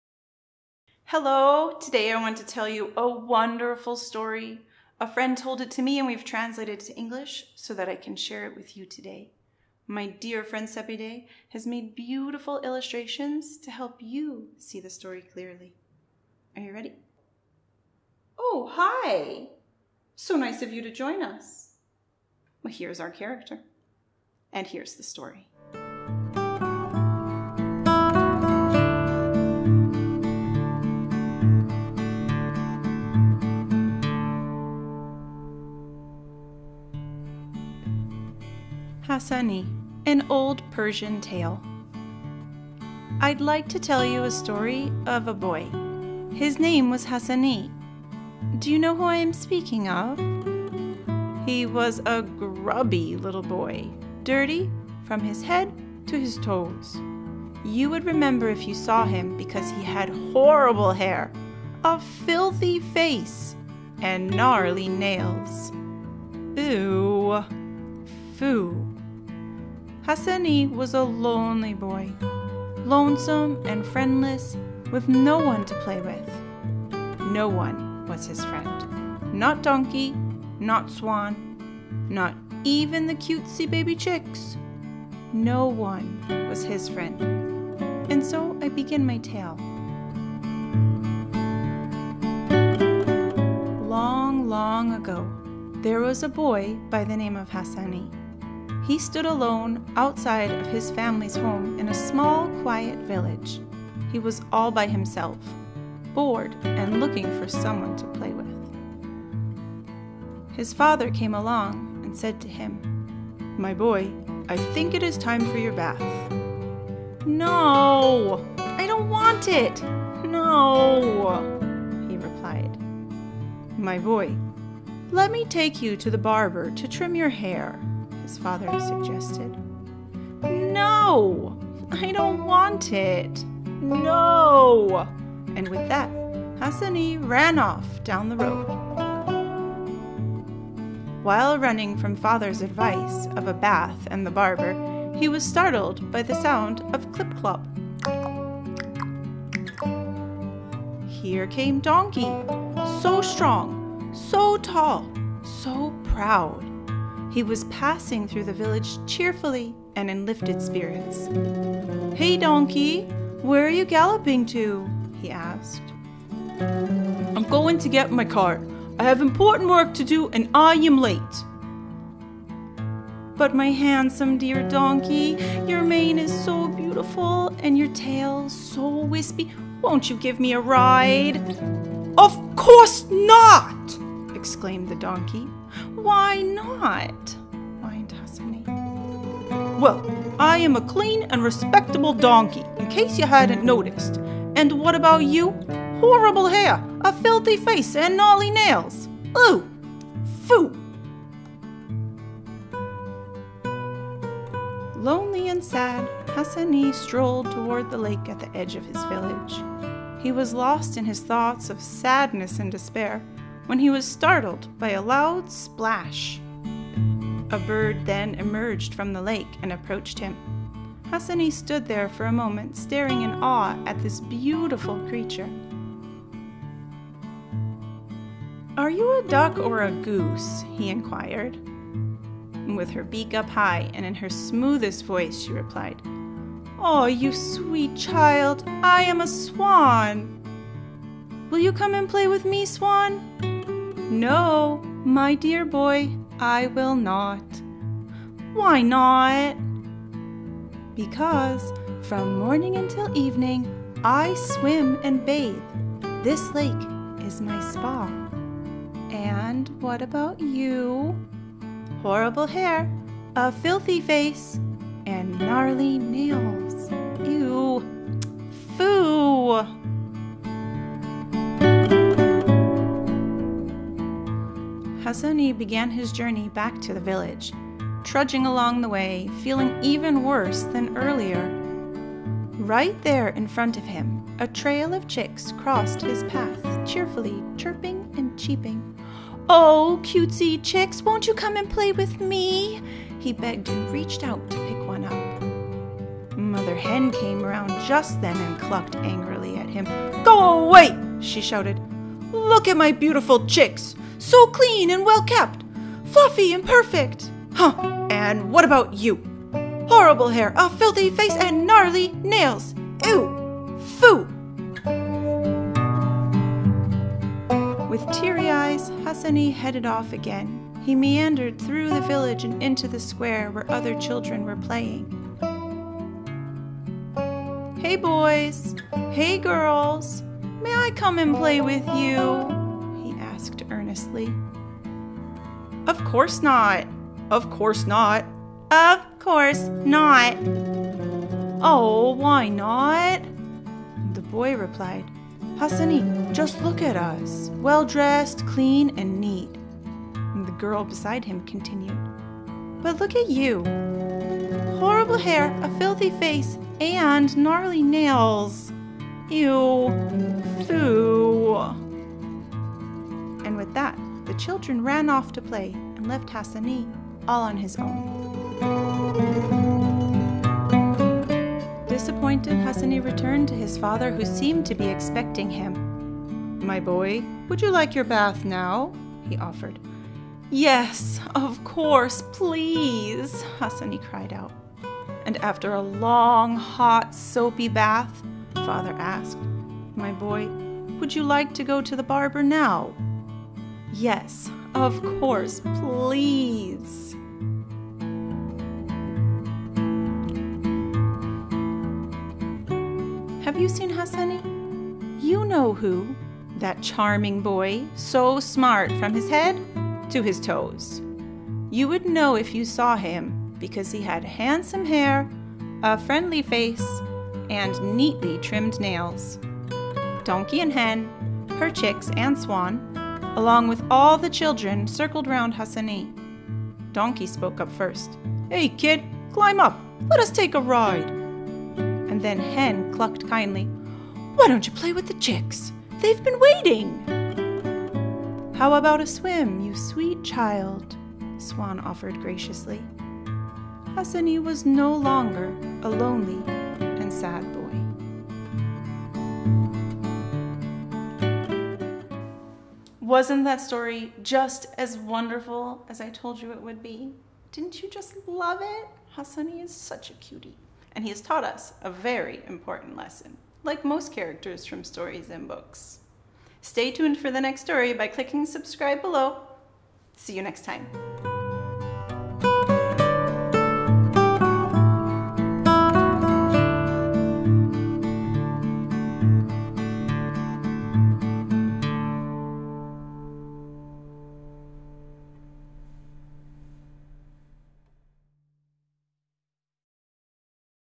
Hassani, an Old Persian Tale
Here is a beautifully illustrated narration of a famous old Persian tale, Hassani. The boy in the story learns an important lesson after many trials. Take a few minutes and follow his adventure through his village.

Enjoy the magic of storytelling and build literacy skills by engaging in visualisations and exploring story elements.
CREDIT:

Narrator: Sativa Ertola
Illustrator: Sepideh Baratian
Director: Arash Davar

PERMISSION:

All materials including illustrations, narration and script, may not be used or reproduced without explicit written permission from Our Yellow Bench.